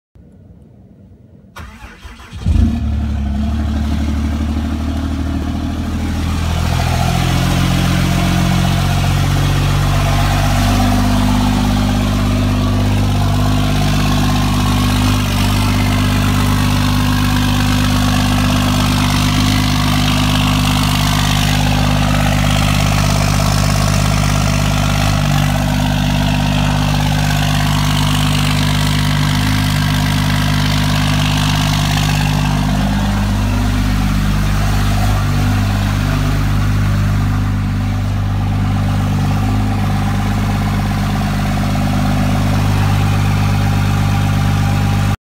4th gen cummins w/ 2nd gen manifold swap cold start 5" exhaust
2012 cummins with a 2nd gen manifold swap
5" straight pipe
fass 150
s465 HTT
HTT 2nd gen manifold
20 degree cold start